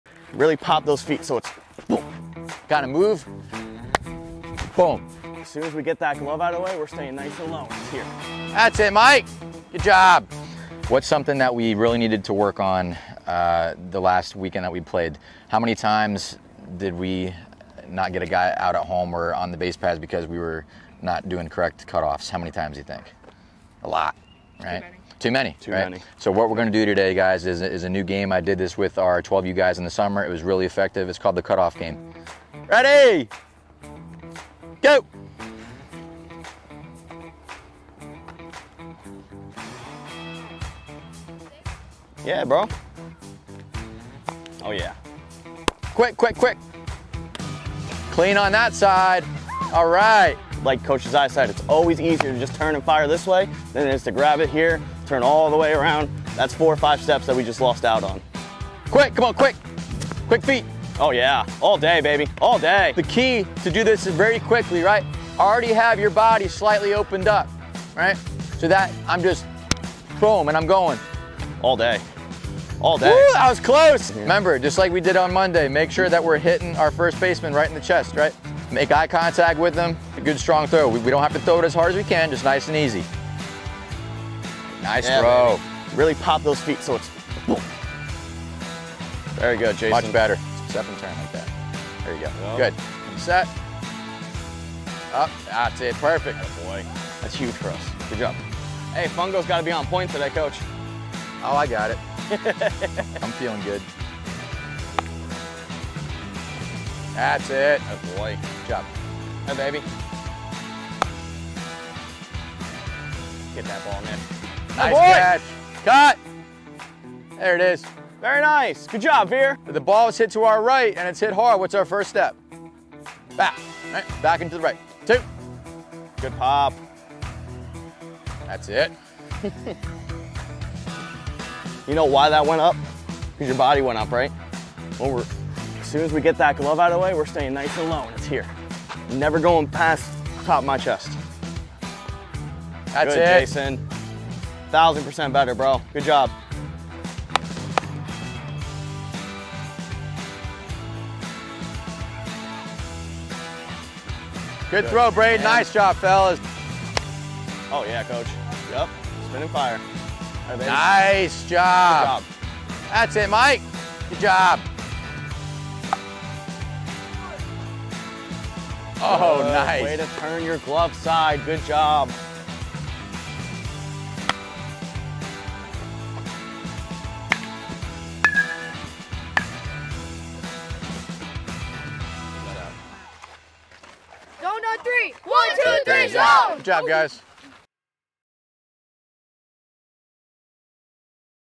COACHES NEED TO TALK TO THEIR PLAYERS LIKE THIS!
Coaches Josiah McBride and Jordan Jones practice relays, pick-offs, and situational fielding with the Zoned RedHawks 12u players.

►Where to follow and listen to Zoned Sports Academy